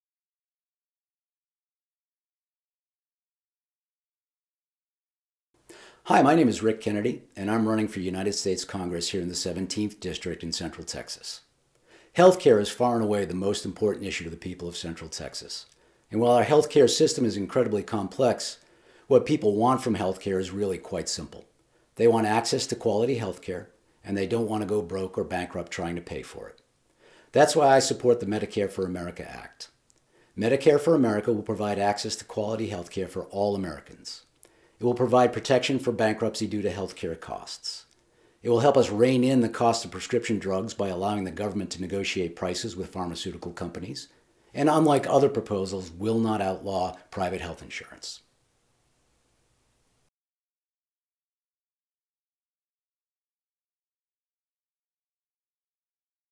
Rick Kennedy for TX17 discusses health care
Here Rick Kennedy discusses the state of healthcare within the TX 17th.